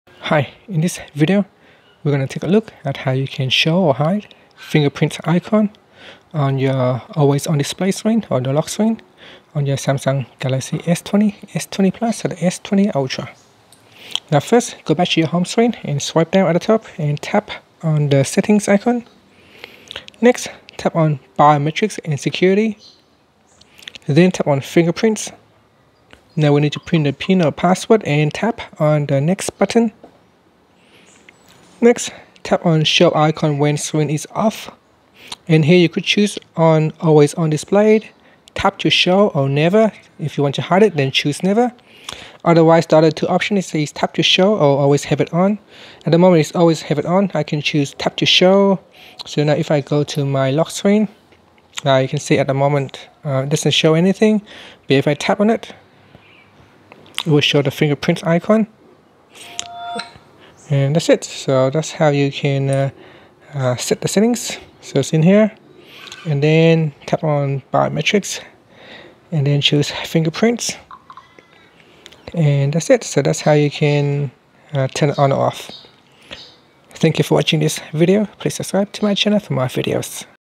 Galaxy S20/S20+: How to Show / Hide Fingerprint Icon on Lock Screen
Learn how you can show or hide fingerprint icon on lock screen on Galaxy S20 / S20 Plus / S20 Ultra.

Android 10.